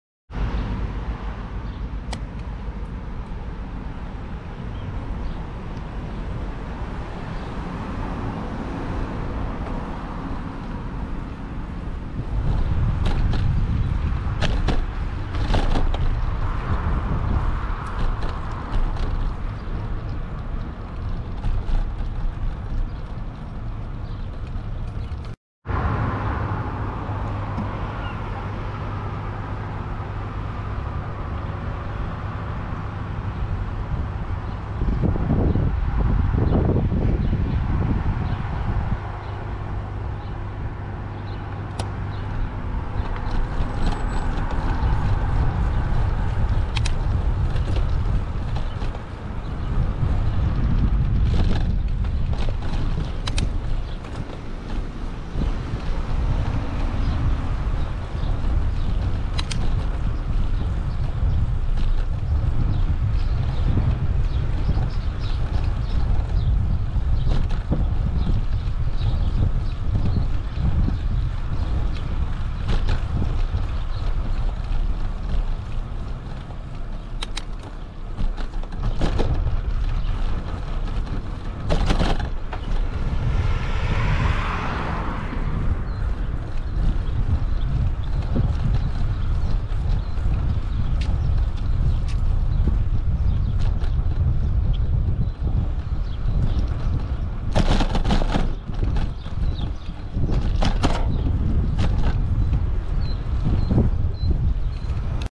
Cottage Grove Road Dempsey Bike Diagonal Detection
Crossing Cottage Grove Road from Dempsey using the bike diagonal signal, along the Lake Loop.